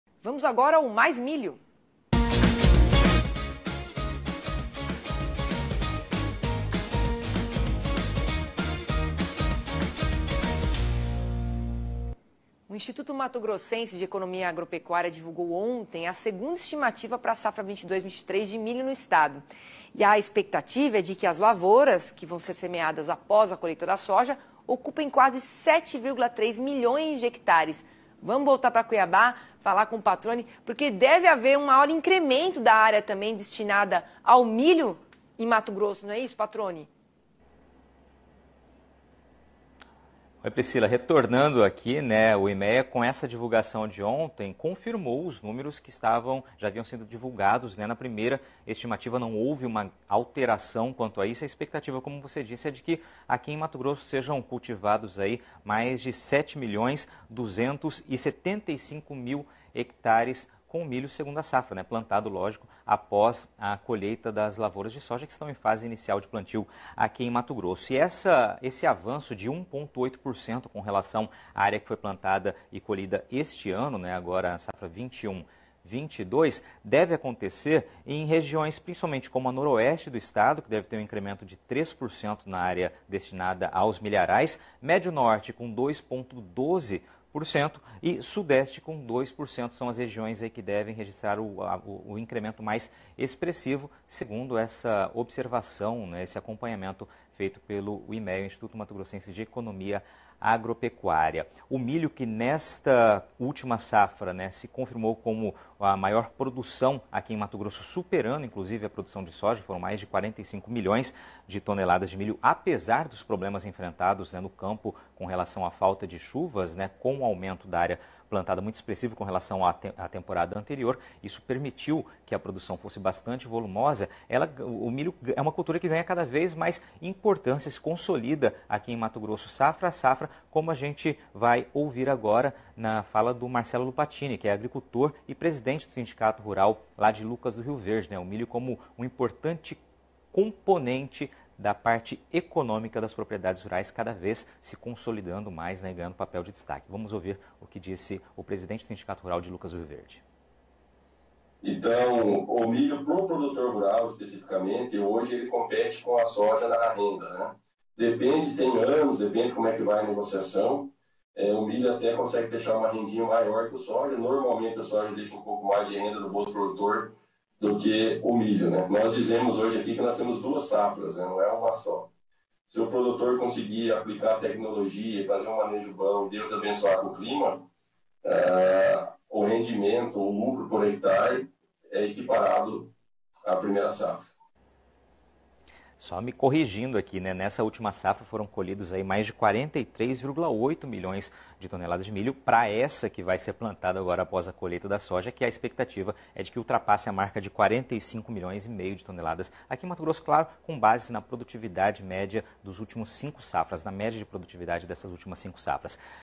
Mais Milho | Lavouras de milho devem ocupar 7,2 milhões de hectares em Mato Grosso | Canal Rural
Dados do Imea projetam aumento de 1,8% na área destinada ao cultivo do cereal na segunda safra no estado.